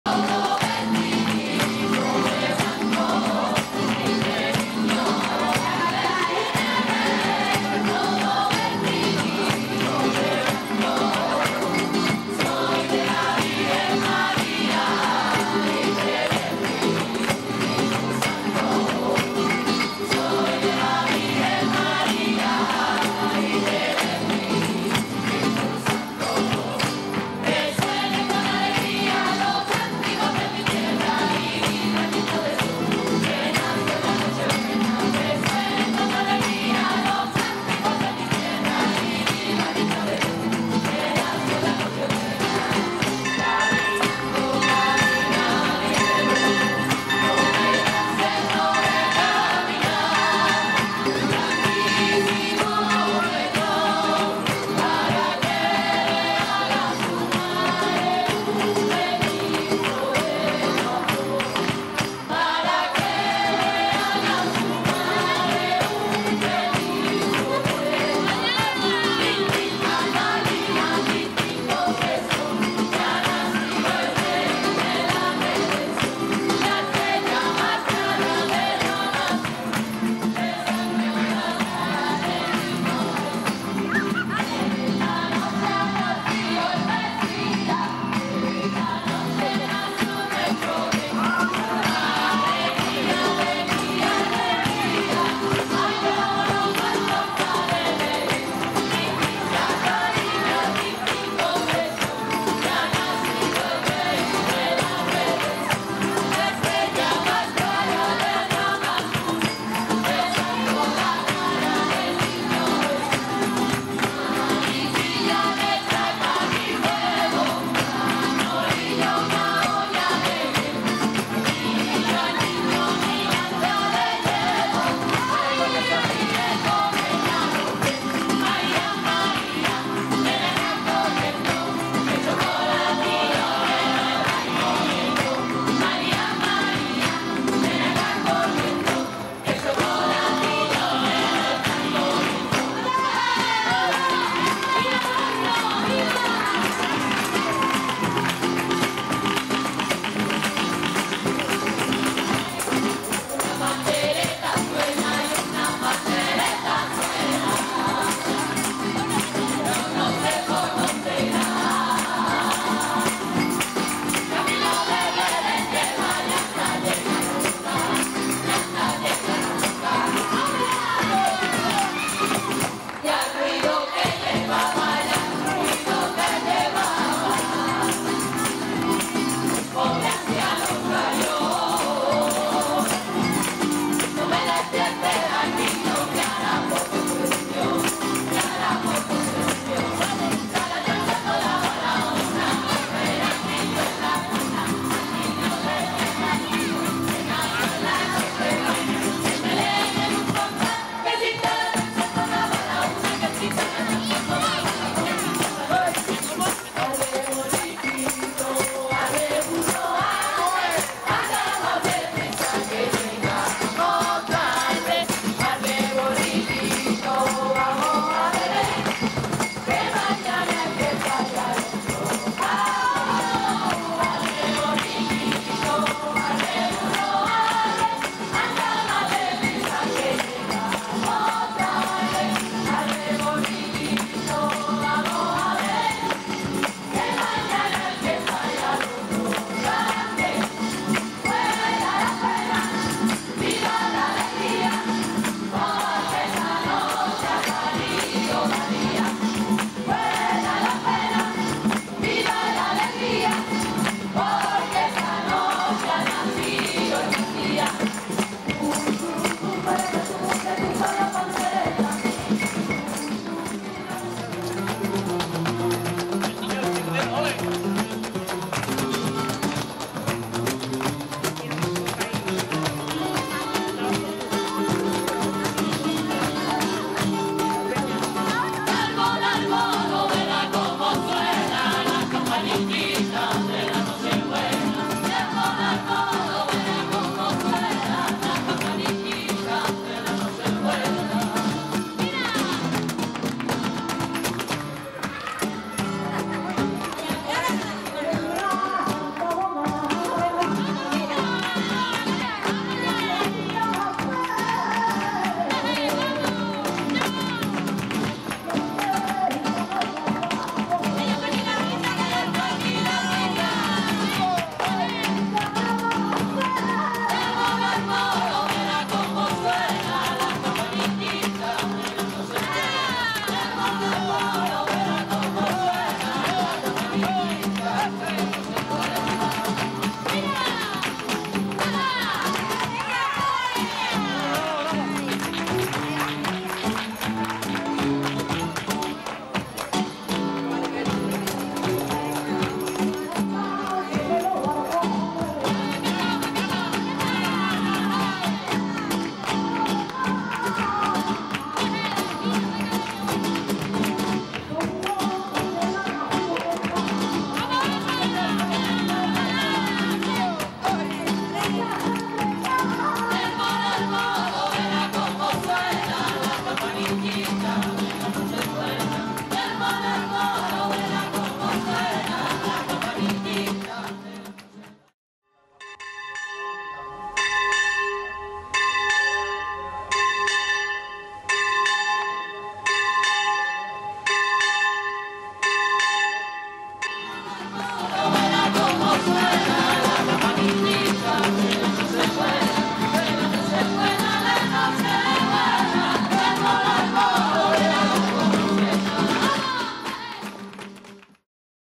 desde el campanario, NACIMIENTO EN LA RESURRECCION FELIZ NAVIDAD & M,AVION
VISITA AL BELEN INSTALADO EN LA RESURRECCION BORNOS NAVIDAD 2010